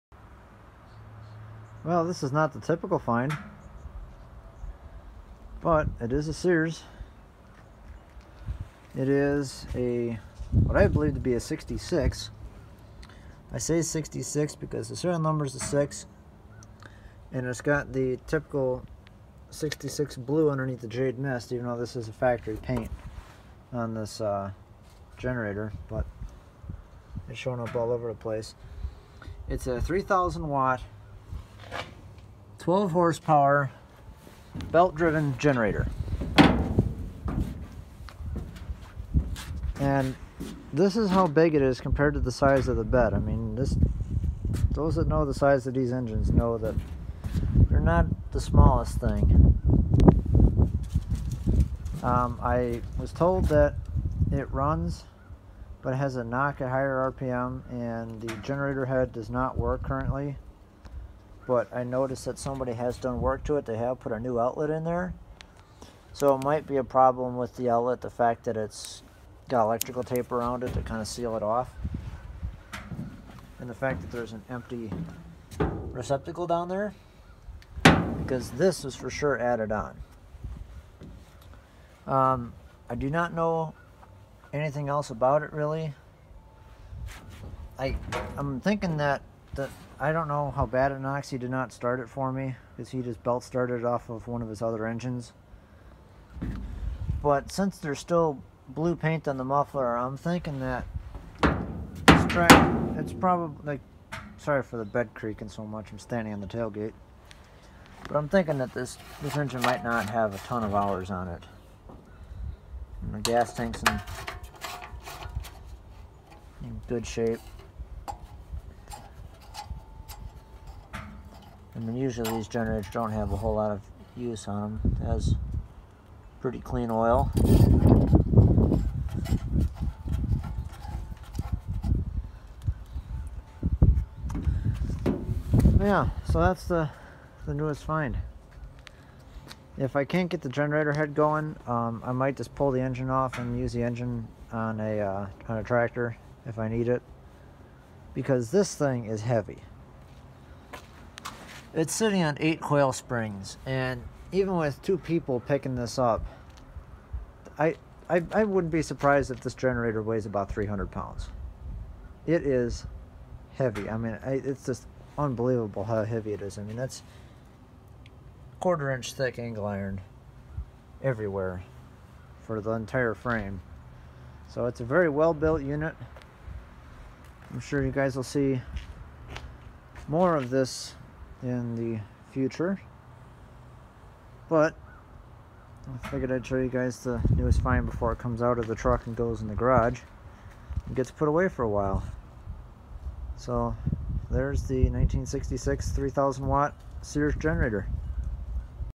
1966 Sears 3000 Watt Generator
This generator is powered by a Tecumseh HH120 and belt driven to a 3000 watt generator head made by Generac. It was very unusual so it came home with me!